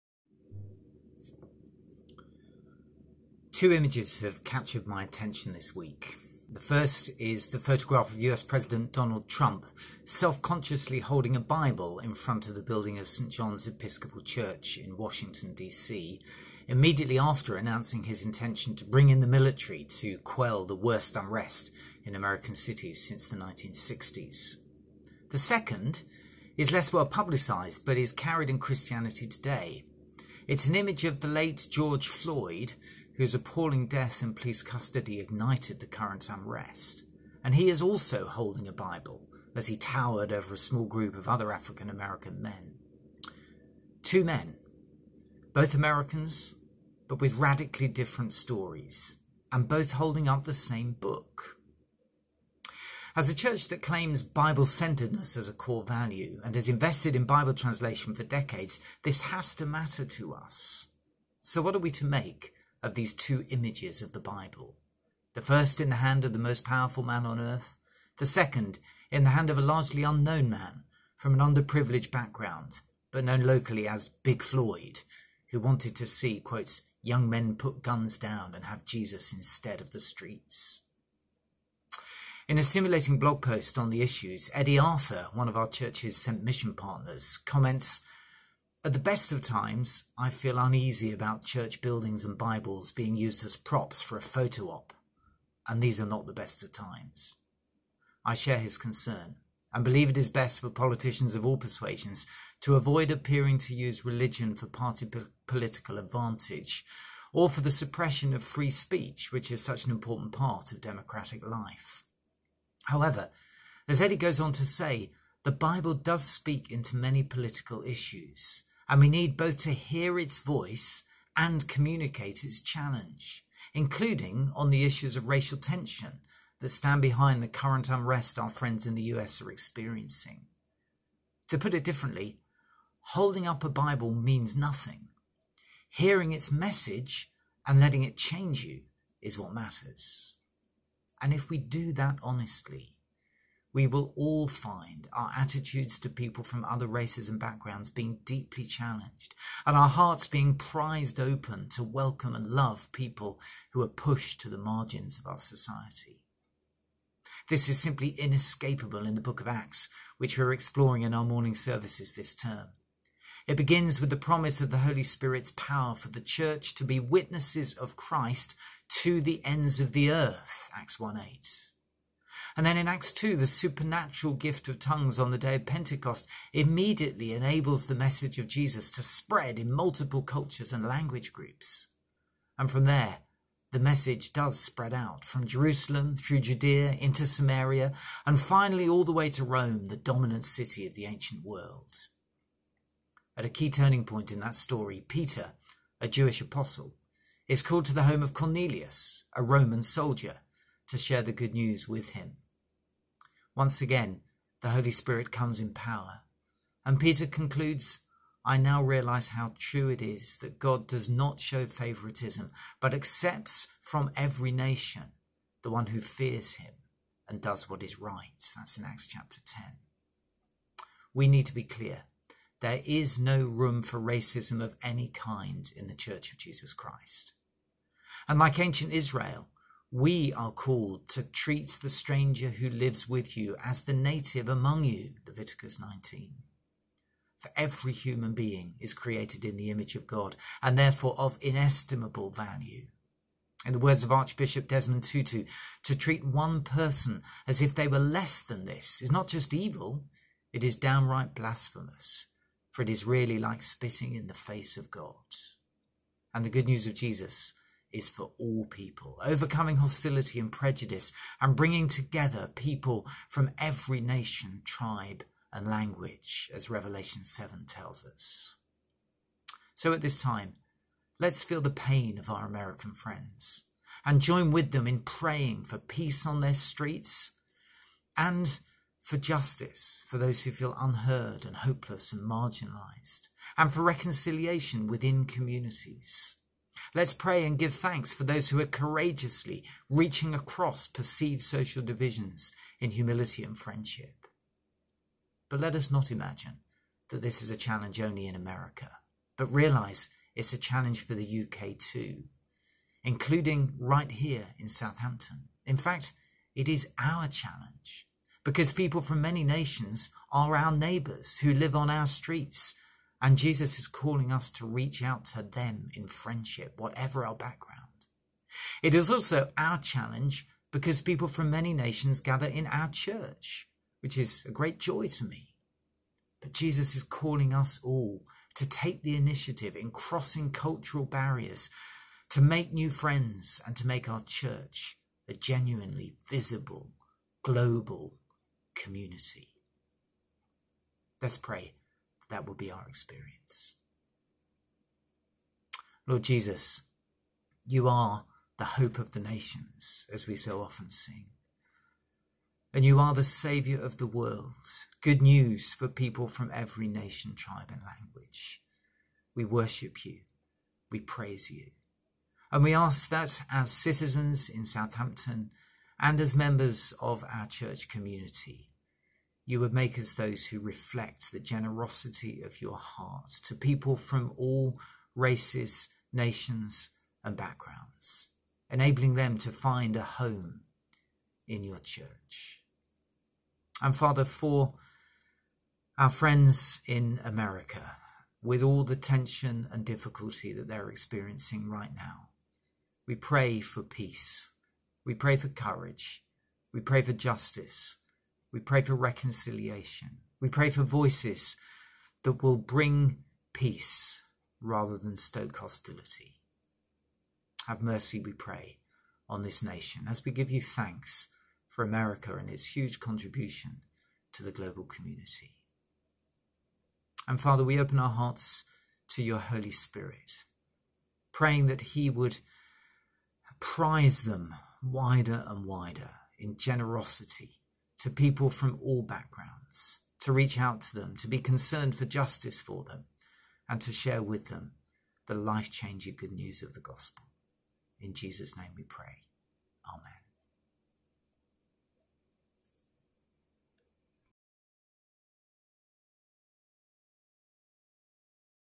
Daily Devotion // 4th June // John Risbridger
Transcript below:

Two images have captured my attention this week.

The first is the photograph of US President Donald Trump, self-consciously holding a Bible in front of the building of St John’s Episcopal Church in Washington, immediately after announcing his intention to bring in the military to quell the worst unrest in American cities since the 1960s.

The second is less well-publicised but is carried in Christianity Today. It is of the late George Floyd (whose appalling death in police custody ignited the current unrest) also holding a Bible as he towered over a small group of other African-American men: two men with radically different stories; both holding up the same book.

As a church that claims ‘Bible-centredness’ as a core value and has invested in Bible translation for decades, this has to matter to us. So what are we to make of these two images of the Bible – the first in the hand of the most powerful man on earth; the second in the hand of a largely unknown man from an under-privileged background, but known locally as ‘Big Floyd’ who wanted to see ‘young men put guns down and have Jesus instead of the streets’ ?

In a stimulating blog post on the issues, Eddie Arthur, one of our church’s sent mission partners, commented “At the best of times, I feel uneasy about church buildings and Bibles being used as props for a photo op – and these are not the best of times.” I share his concern and believe it is best for politicians of all persuasions to avoid appearing to use religion for party political advantage, or the suppression of free speech, which is such an important part of democratic life.
However, as Eddie goes on to say, the Bible does speak into many political issues and we need both to hear its voice and communicate its challenge – including on the issues of racial tension, that stand behind the current unrest our friends in the US are experiencing. To put it differently: holding up a Bible means nothing; hearing its message, and letting it change you is what matters. And if we do that honestly, we will find all our attitudes to people from other races and backgrounds being deeply challenged, and our hearts prized open to welcome and love people who are pushed to the margins of society.

This is simply inescapable in the book of Acts which we are exploring in our morning services this term. It begins with the promise of the Holy Spirit’s power for the church to be witnesses of Christ to the ends of the earth (Acts 1:8). The supernatural gift of tongues on the day of Pentecost immediately enables the message of Jesus to spread in multiple cultures and language groups (Acts 2:5-8). And from there the message spreads from Jerusalem, through Judea, into Samaria, and finally all the way to Rome, the dominant city of the ancient world.

At a key turning point in that story, Peter, a Jewish Apostle, is called to the home of Cornelius, a Roman soldier, to share the good news with them. Once again the Holy Spirit comes in power and Peter concludes, “I now realise how true it is that God does not show favouritism but accepts from every nation the one who fears him and does what is right.” (Acts 10:34-5).

We need to be clear: there is no room for racism of any kind in the church of Jesus Christ and, like ancient Israel, we are called to “treat the stranger who lives with you as the native among you” (Lev 19:34). For every human being is created in the image of God and therefore of inestimable value. In the words of Archbishop Desmond Tutu, “To treat one person as if they were less than this is not just evil… it is downright blasphemous… for it is really like spitting in the face of God.” And the good news of Jesus is for all people, overcoming hostility and prejudice and bringing together people ever nation, tribe and language (Rev 7:9).

So let’s feel the pain of our American friends and join with them in praying for peace on the streets of America – and for justice for those who feel ‘unheard’, hopeless and marginalised, and for reconciliation within communities. Let’s pray and give thanks for those who are courageously reaching across perceived social divisions, in humility and friendship. But let us not imagine that this is a challenge only in America, but realise that it is a challenge for the UK too – including right here in Southampton. In fact it is our challenge because people from many nations are our neighbours who live on our streets and Jesus is calling us to reach out to them in friendship, whatever our background. It is also our challenge, because people from many nations gather in our church, which is such a joy to me, and Jesus is calling us all to take the initiative in crossing cultural barriers, make new friends and make our church a genuinely, visibly global community.